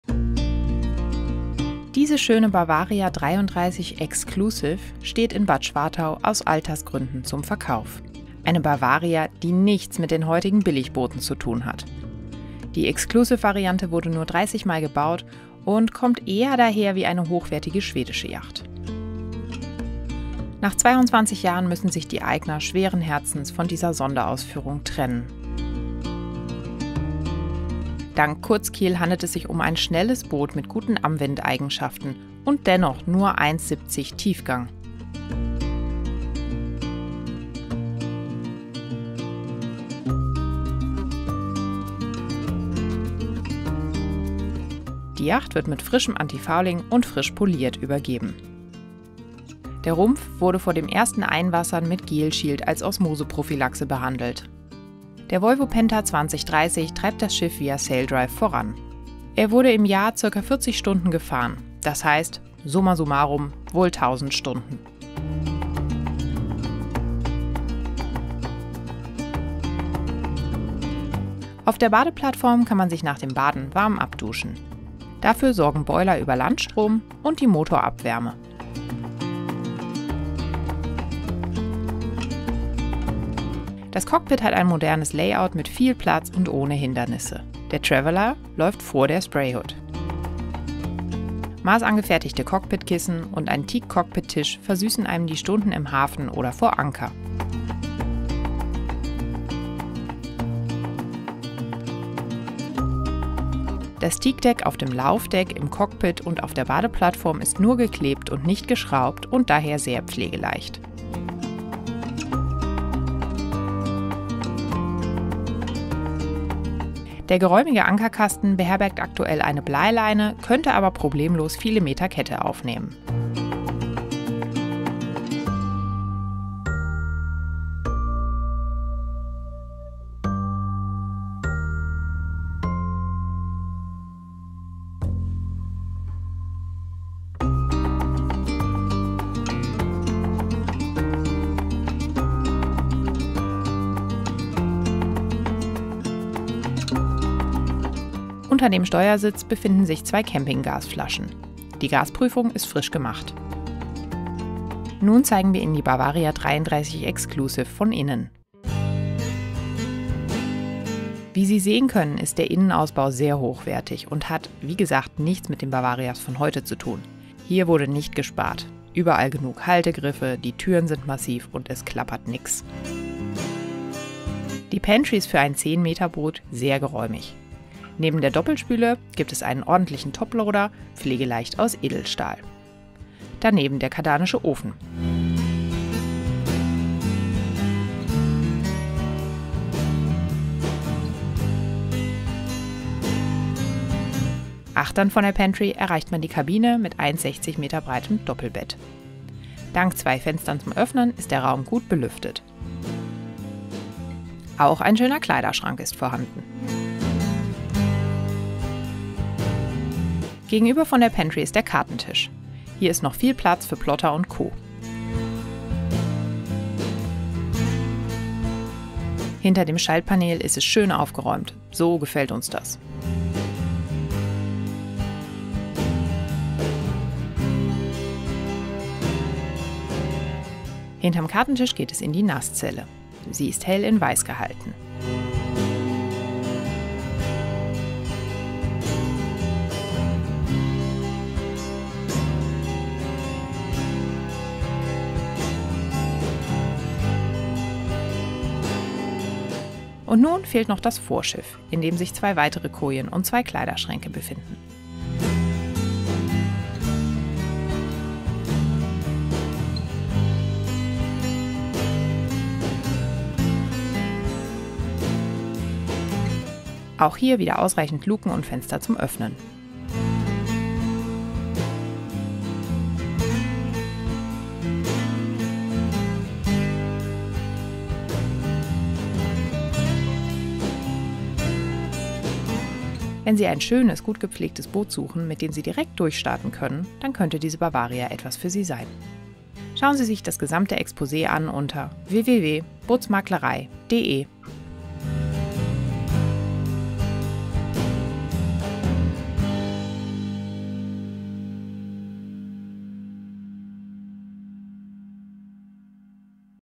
Bavaria 33 exclusive steht zum Verkauf - Rundgang durch die Segelyacht (VERKAUFT)
Liegeplatz ist Bad Schwartau
Bootsmaklerei

Einen Einblick hinter die Kulissen der Bootsmaklerei geben wir auf Facebook und Instagram:

00:00 Aussenansicht
01:16 Innenansicht

Alle Rechte am Bild bei der Bootsmaklerei
Einverständniserklärungen der abgebildeten Personen liegen vor!